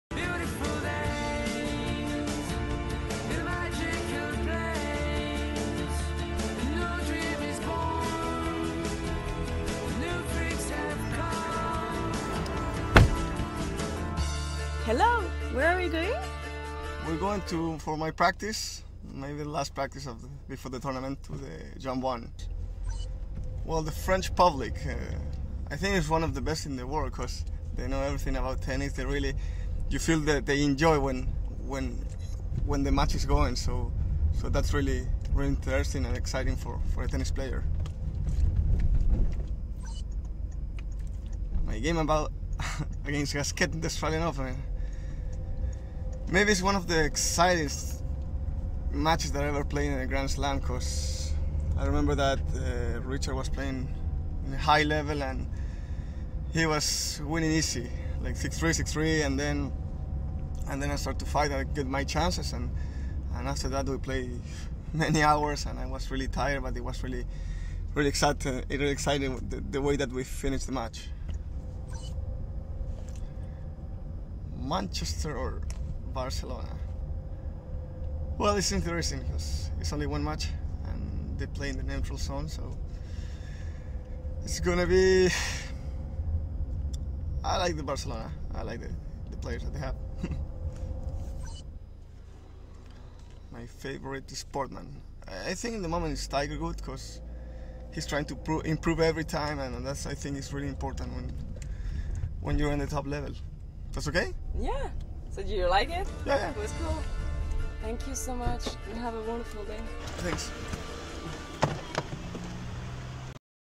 Fernando Gonzalez In Car interview! RG
Fernando Gonzalez gets answers random questions while on his way to practice. =D